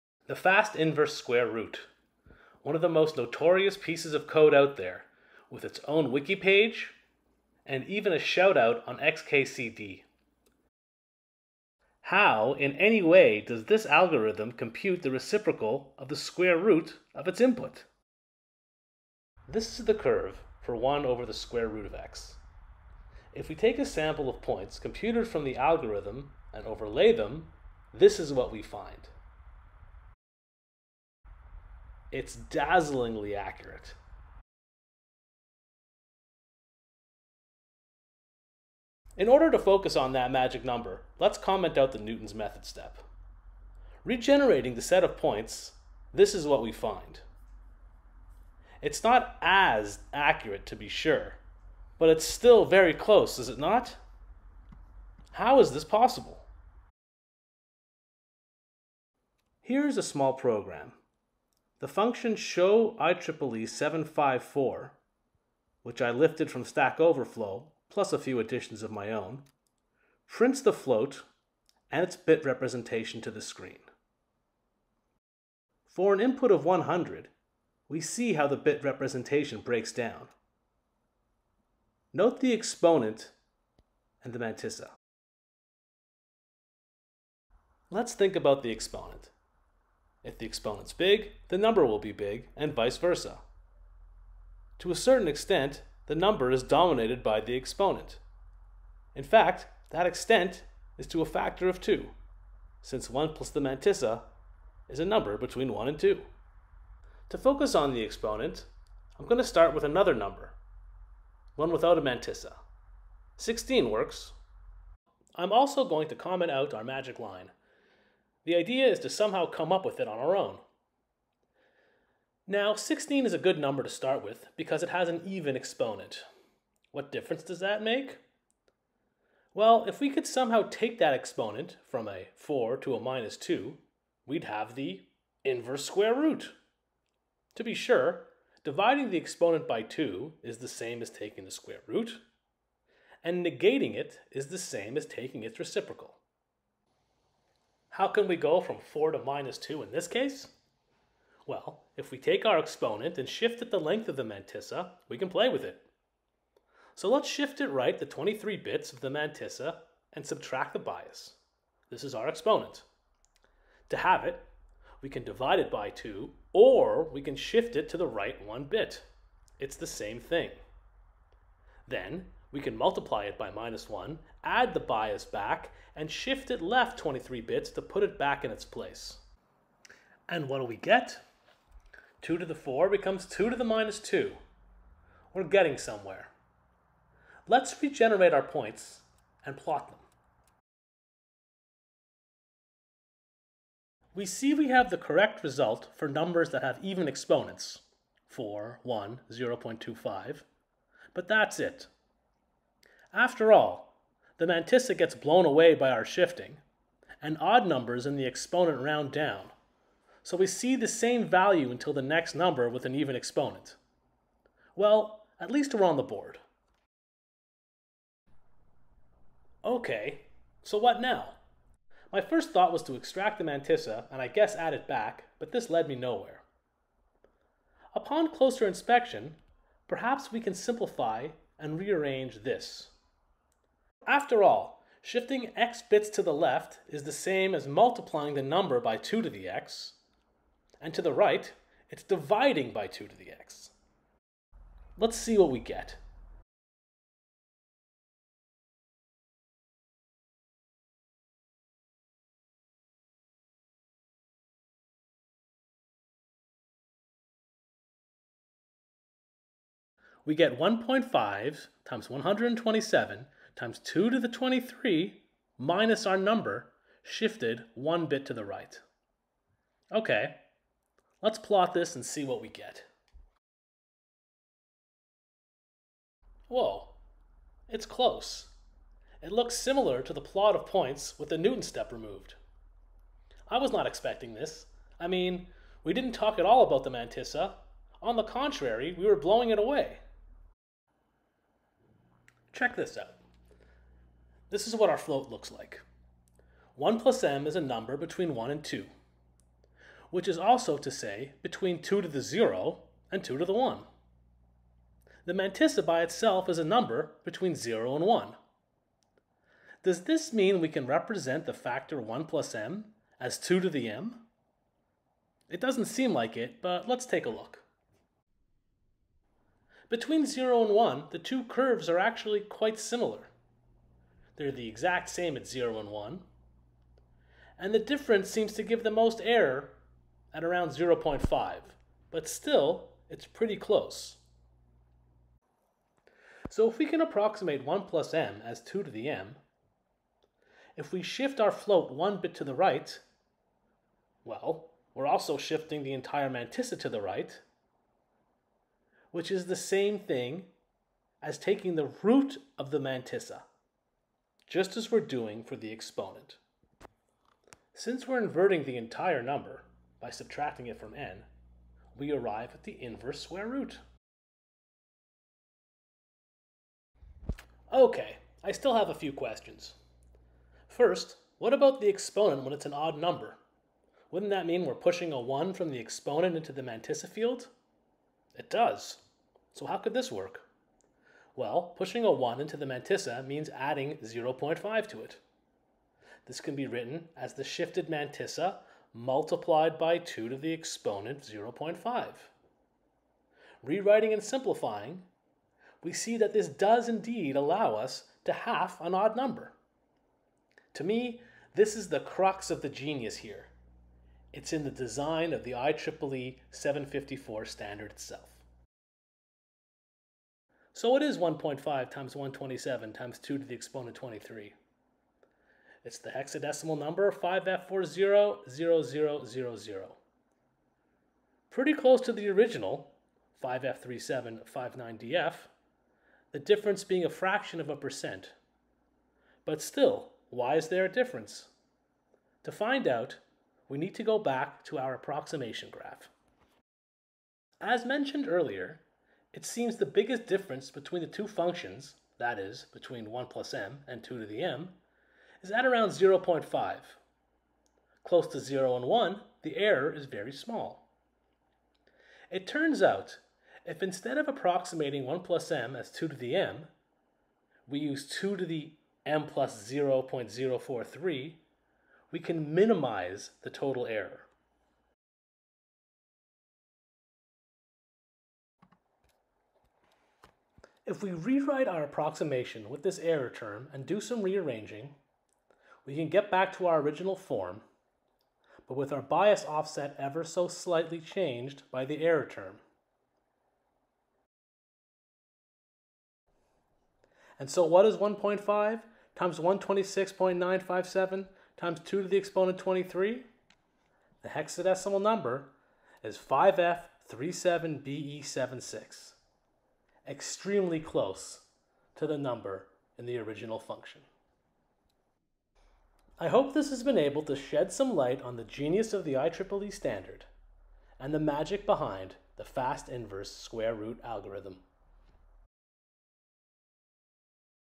The Fast Inverse Square Root -- 0x5f3759df explained!!
The Fast Inverse Square Root is one of the most notorious code listings out there.  I hope this video sheds some light on the magic behind it!

I assume the viewer has some familiarity with the code.